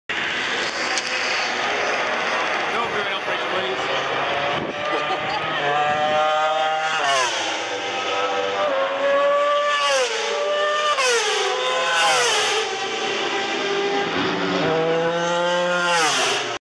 Silverstone 2013
A quick vid from Silverstone 2013 Practice and Qualifying